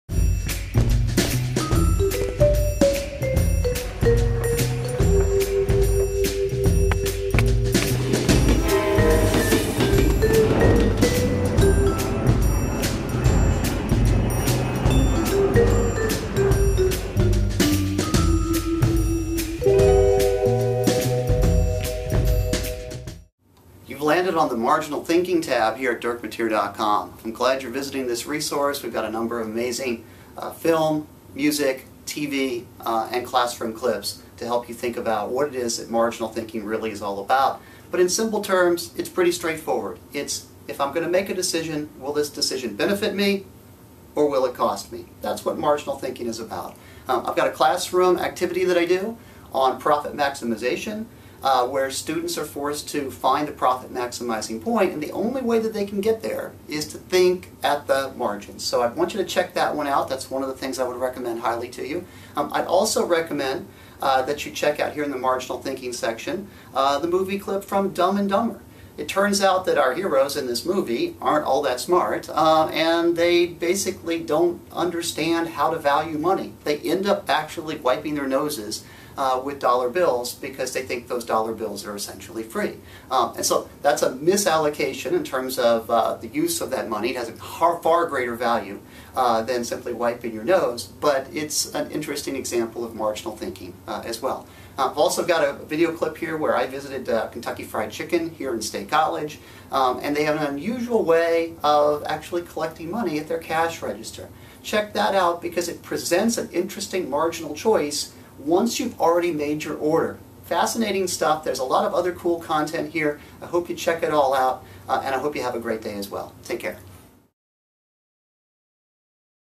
Marginal Thinking Topic Overview
An introduction to the economic theory of Marginal Thinking.